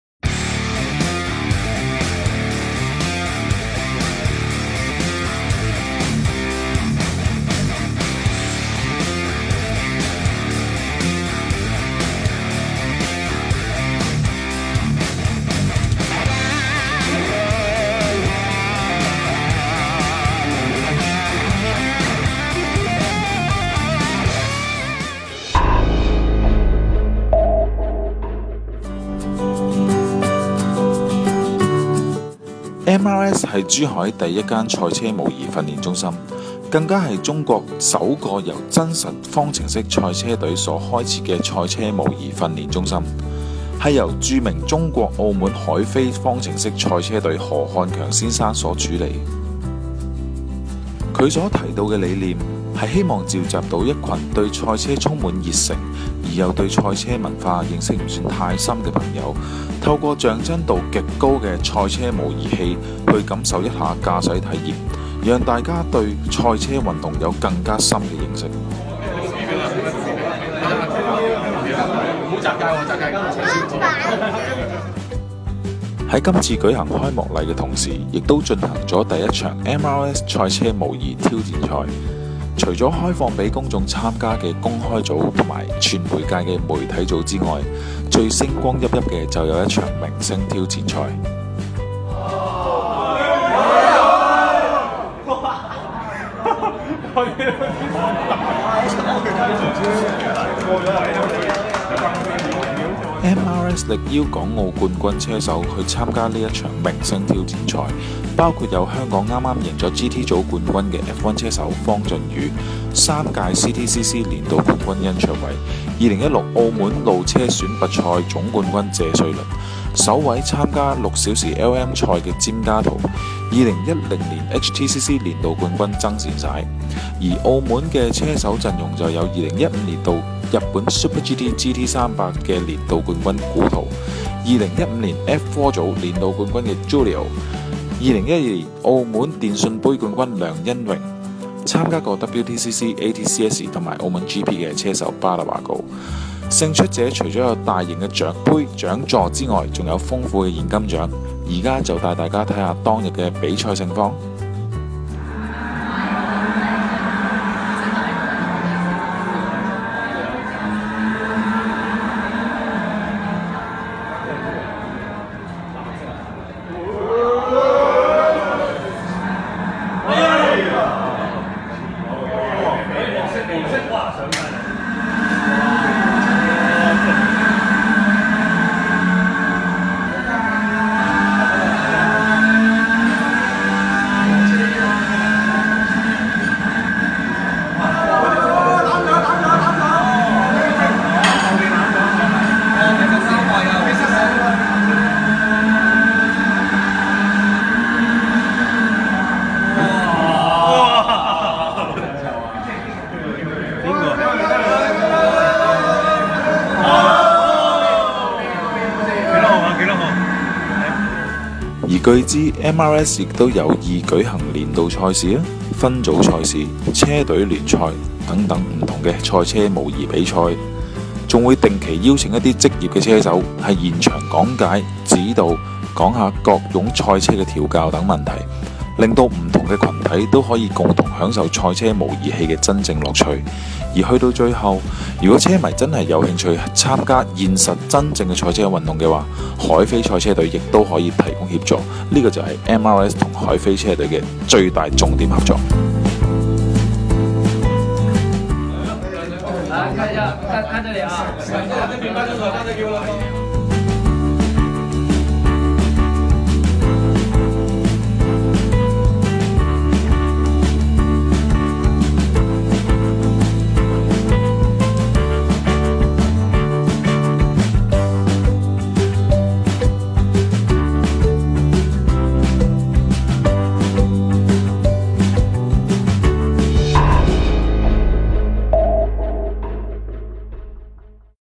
【MRS賽車模擬訓練中心正式開業】
MRS Moto Racing Simulator 邁進賽車模擬訓練中心，係中國首個由真實方程式賽車隊所開設的賽車模擬訓練中心，由著名
Champ Motorsport凱飛方程式賽車隊Michael Ho先生所主理。他貫徹理念，希望一些對賽車充滿熱誠的人仕可以透過像真度極高的賽車模擬器去感受一下駕駛體驗。
是次開幕禮，配合首屆【MRS模擬賽車挑戰賽】，更邀請了香港F1車手Adderly Fong - 方駿宇、CTCC三屆年度冠軍Andy Yan 甄卓偉、2016路車選拔賽總冠軍謝瑞麟、首位參加6小時LM賽詹家圖 （Ka To Jim）、2010 HTCC年度冠軍曾善璽(Billy Tsang)。澳門車手陣容就有2015日本Super GT300年度冠軍Andre Couto(古圖)、2015 F4年度冠軍Julio Sebastian Acosta、2012澳門電訊盃冠軍Kelvin Leong(梁欣榮)、參加過WTCC、ATCS、MTCS、MacauGP車手JBad Badaraco(巴達華高)。
而MRS亦表示有意舉行年度賽事、分組賽事、車隊聯賽等等不同賽車模擬比賽，更歡迎各大公司及群體包場作賽。更會定期邀請職業車手現場指導及講解賽車調較等問題！